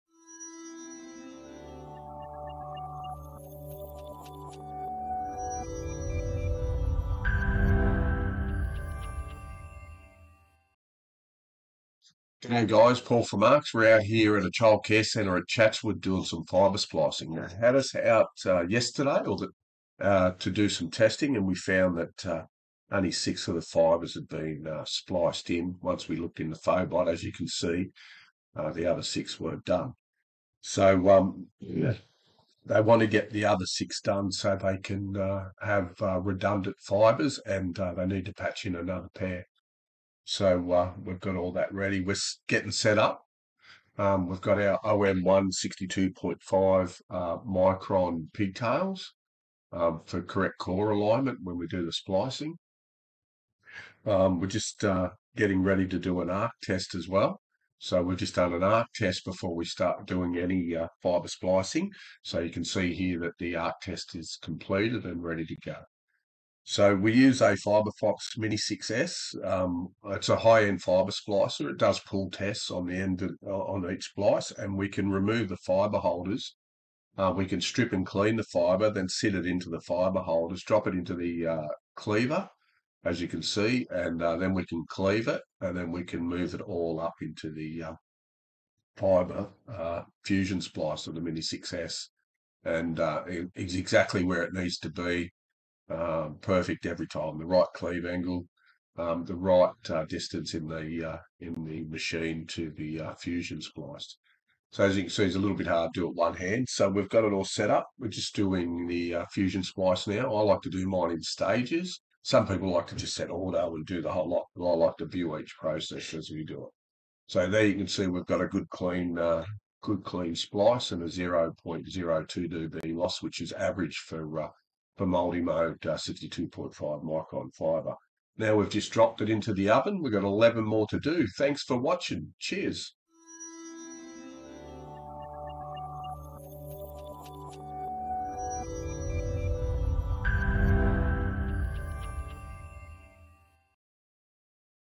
Fiber fusion Splicing OM1 Pigtails
Sorry haven't done a video in a while so I thought I do one,

Here we were requested to help out with some fiber issues at the site in Chatswood. Testing found only 6 cores were spliced out of the 12 cores available.

So we were asked to complete the splicing on the lower ground to the ground floor FOBOTS.

The cable was an OM1  62.5 um micron 12-core cable, so we sourced some matching pigtails for correct core alignment.

Sorry have to voice over fan was noise too loud